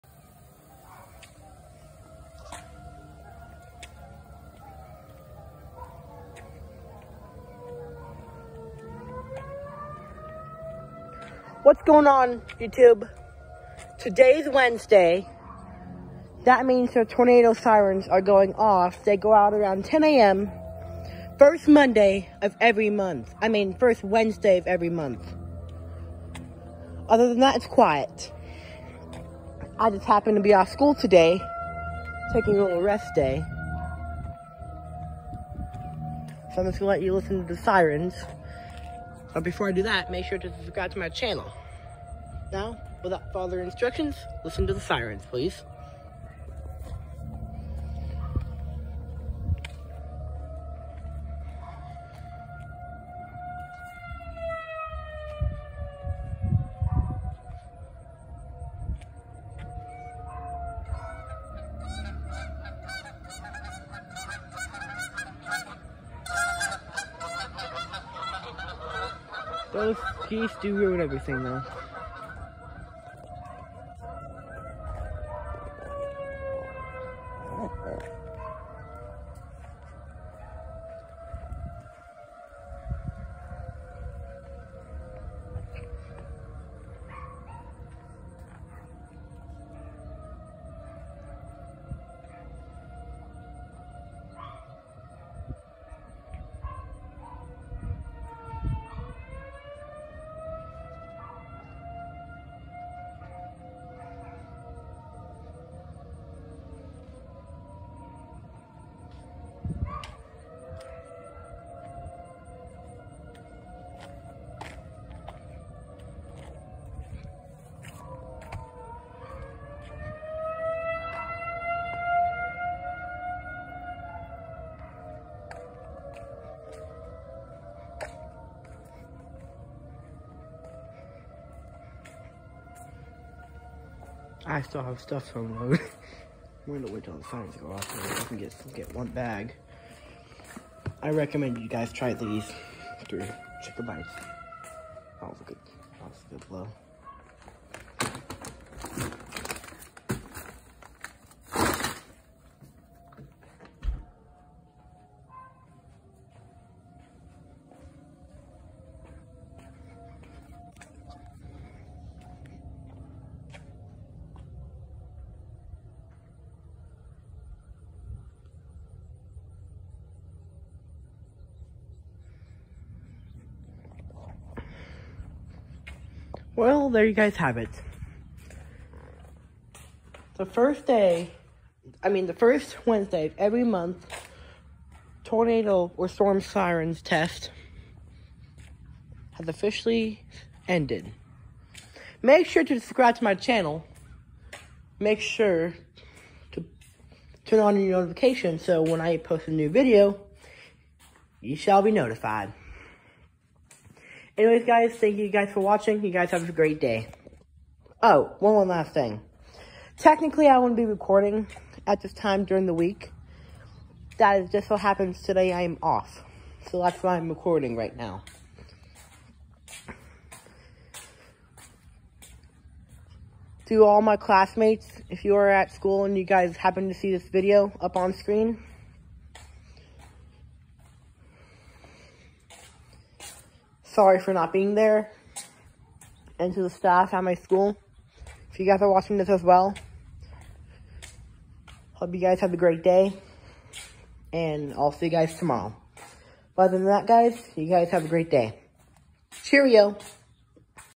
￼ first Wednesday of every month tornado sirens test
Enjoy the sirens they’re only going to get louder and they’re only gonna get better.